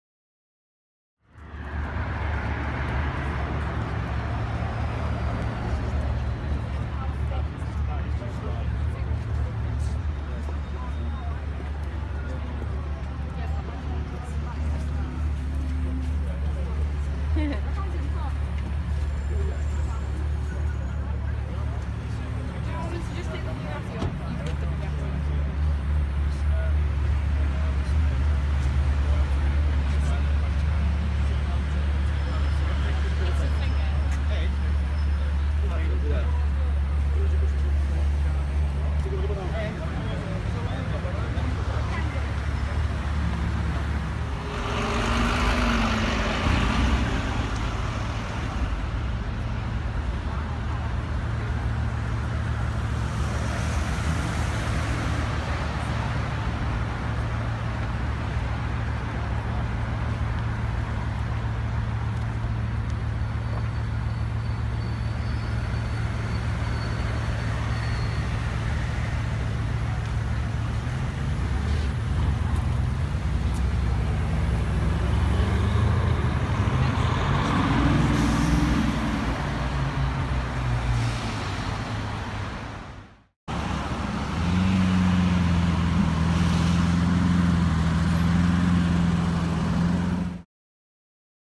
Ferrari LeFerrari or Bugatti Veyron??
Rich millionaire Arab cannot decide which of his hypercars he will be driving around London in tonight!! Liked the video? Thanks :D